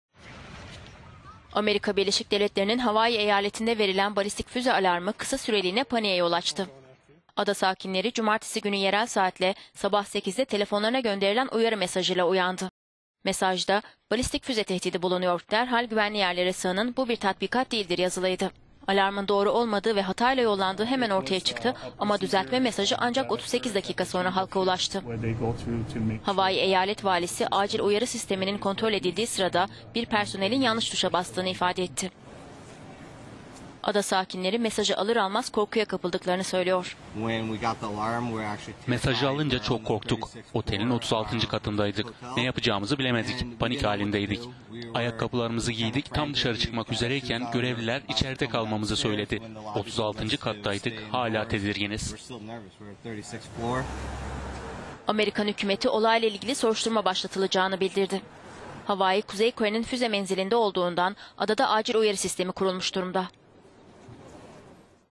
Hawaii'de yanlış füze alarmı
Yanlış tuşa basıp füze uyarısı gönderen personel paniğe yol açtı…